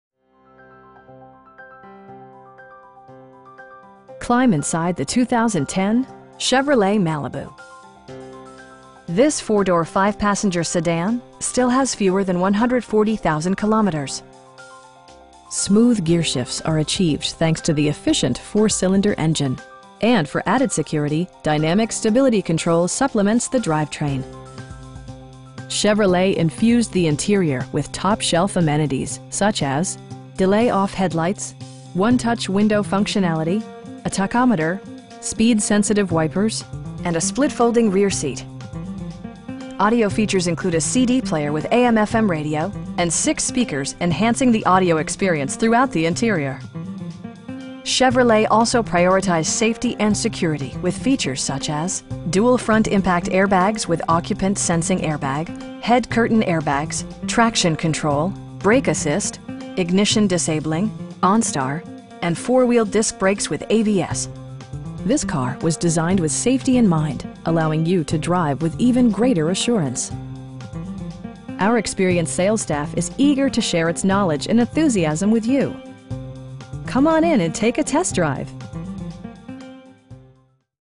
2010 Chevrolet Malibu 2 Years Warranty, Certified
Topbillin Auto Sales
2-7855 Keele St
in Vaughan, ON L4K 1Y6

Climb inside the 2010 Chevrolet Malibu. This 4 door, 5 passenger sedan still has fewer than 140,000 kilometers! Smooth gearshifts are achieved thanks to the efficient 4 cylinder engine, and for added security, dynamic Stability Control supplements the drivetrain. Chevrolet infused the interior with top shelf amenities, such as: delay-off headlights, 1-touch window functionality, a tachometer, speed sensitive wipers, power door mirrors, cruise control, and a split folding rear seat. Audio features include a CD player with AM/FM radio, and 6 speakers, enhancing the audio experience throughout the interior. Chevrolet also prioritized safety and security with features such as: dual front impact airbags with occupant sensing airbag, head curtain airbags, traction control, brake assist, ignition disabling, onStar, and 4 wheel disc brakes with ABS. This car was designed with safety in mind, allowing you to drive with even greater assurance. Our experienced sales staff is eager to share its knowledge and enthusiasm with you. They'll work with you to find the right vehicle at a price you can afford. Come on in and take a test drive!